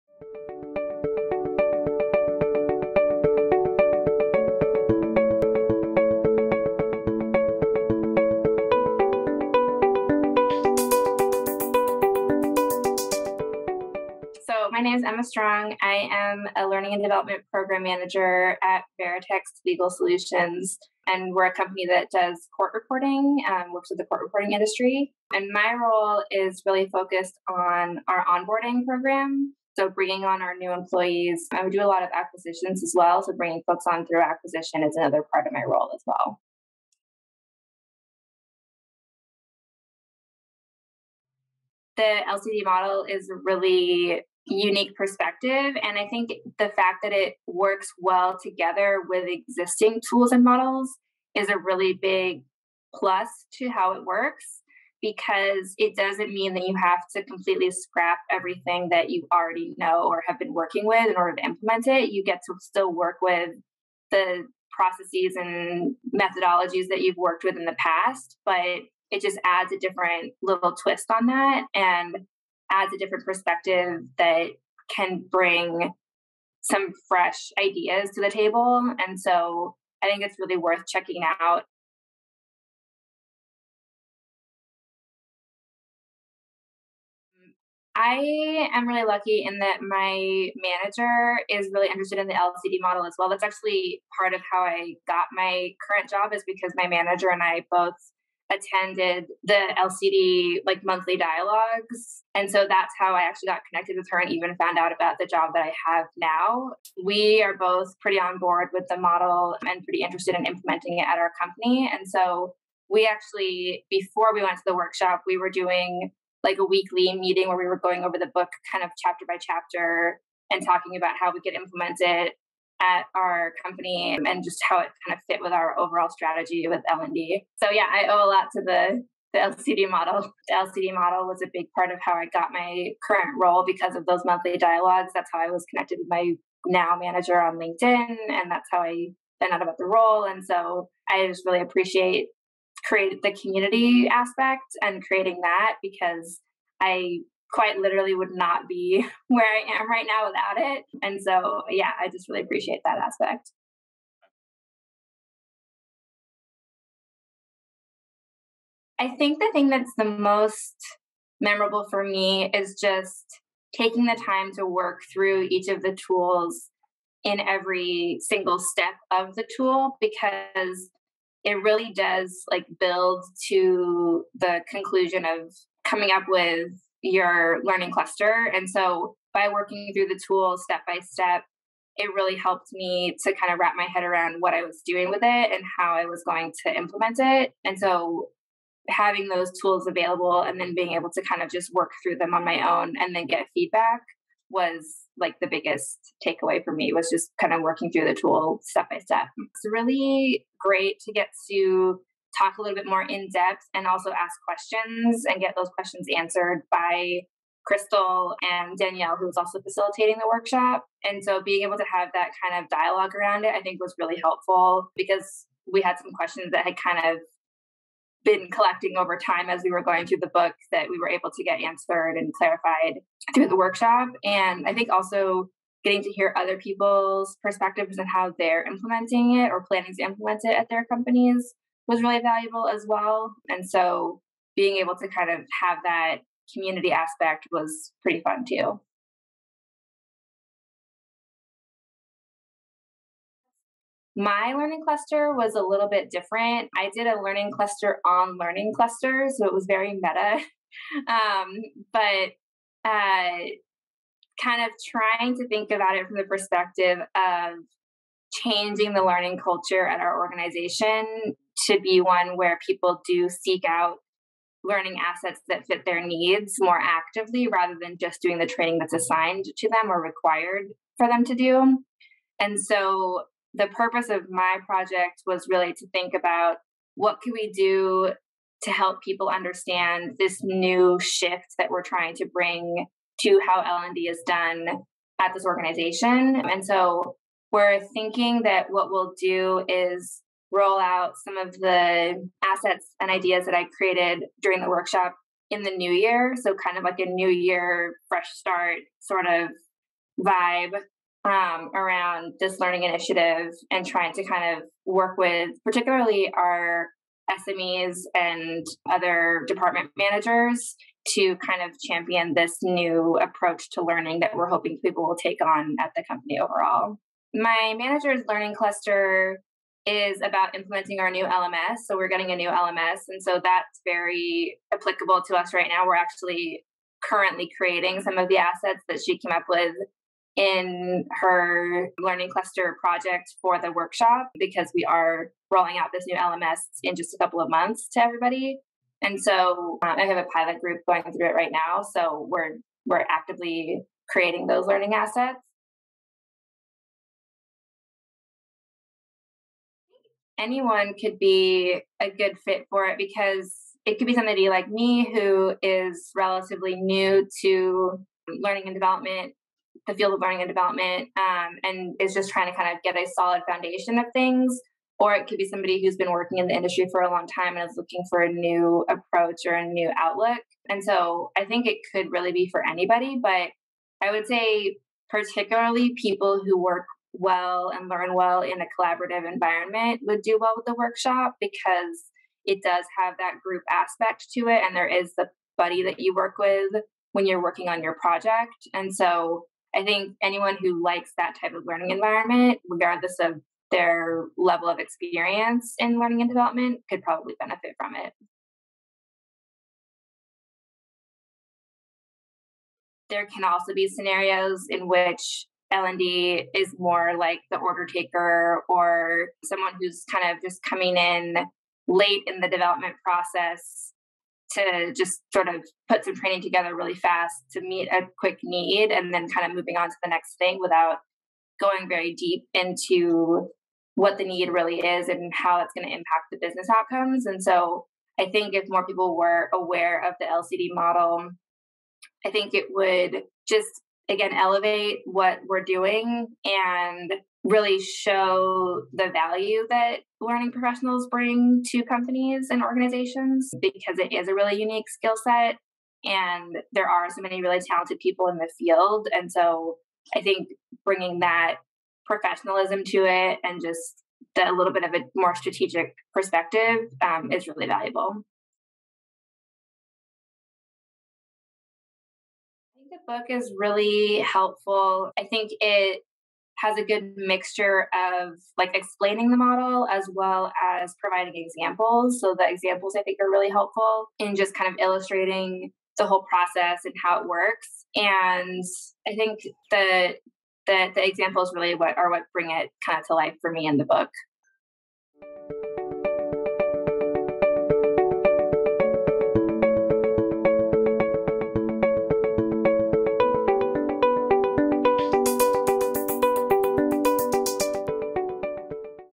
Emma Strong, L&D Manager, Shares Her Experience with the 2-Week Learning Cluster Design Practicum
Emma Strong, L&D manager at Veritext, shares her experience with the Learning Cluster Design 2-week practicum and building a cluster about Learning Clusters.